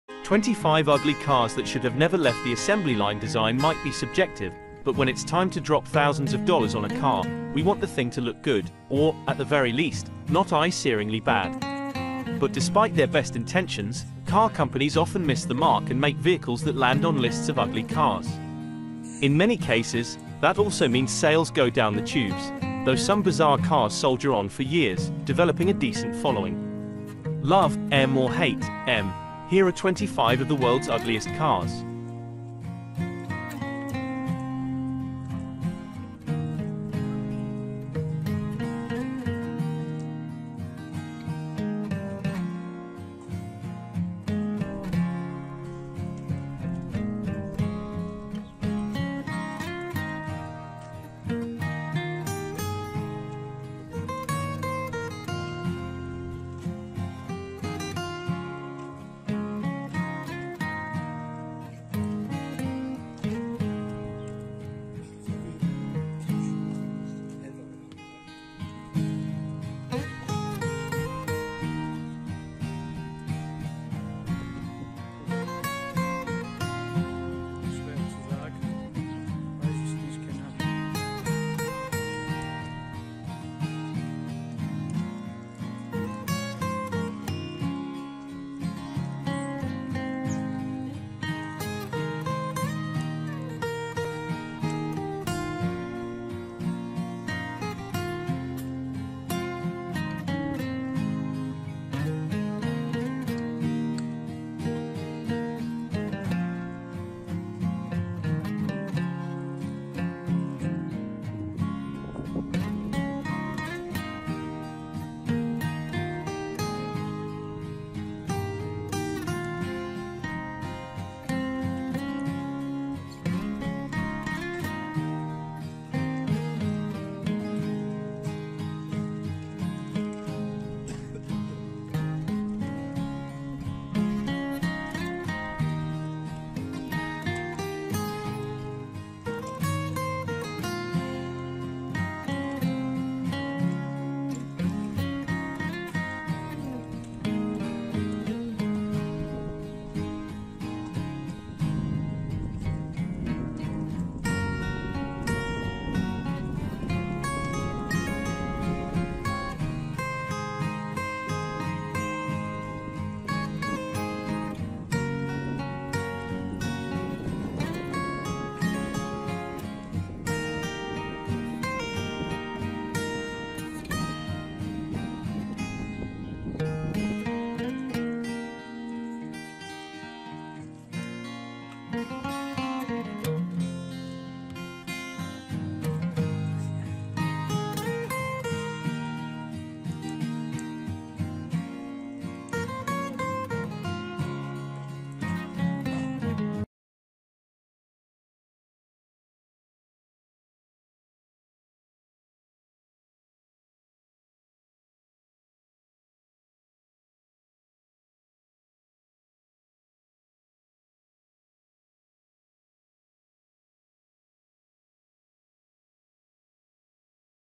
15 Ugly Cars That Should Have Never Left the Assembly Line UGLIEST Cars of the 1970's
15 Ugly Cars That Should Have Never Left the Assembly Line UGLIEST Cars of the 1970's

1970 AMC Gremlin
The Gremlin sold for two-thirds the cost of most small cars, which is appropriate, because it looked like two-thirds of a car. Truth be told, the Gremlin was a stroke of corporate genius, cheap to develop and right for the times, and it was a runaway sales success, at least by AMC's modest standards. But did it have to be so ugly? As we outlined in our brief history of the Gremlin, it could have been worse: AMC considered a shortened version of the Hornet that design chief Dick Teague said was even uglier—though considering that he signed off on the Gremlin, we're not sure his opinion can be trusted.

I do NOT own some or all of the video materials used in this video.